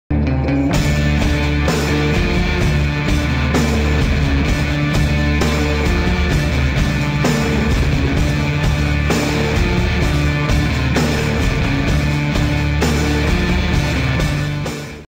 Water trolley 220m Khabarovsk
Наш самый длинный веревочный троллей над водой.
Команда DROPROPE
droprope.ru